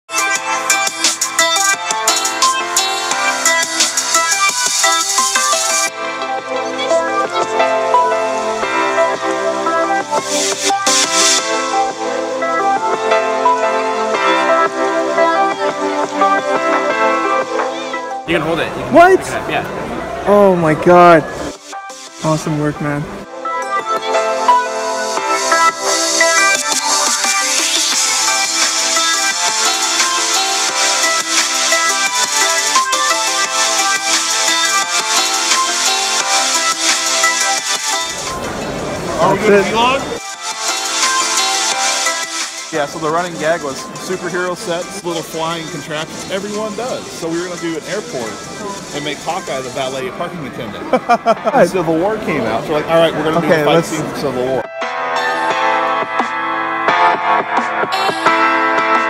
BRICKCON2016: Part two- October 1, 2016
Good evening! Here's another installment for my Brickcon2016 series. Here's second part of brickcon. Met some incredible builders like Eric(huge waterfall), Taylor with his joker bust, and Jeremy from Portland with his Civil war collab build! Please enjoy these awesome moments that I caught while at brickcon! 😊

With Jeffrey Allen Tom, Colleen Rodolfo and Ethan Biig-e Woodham.

Please enjoy the VLOG!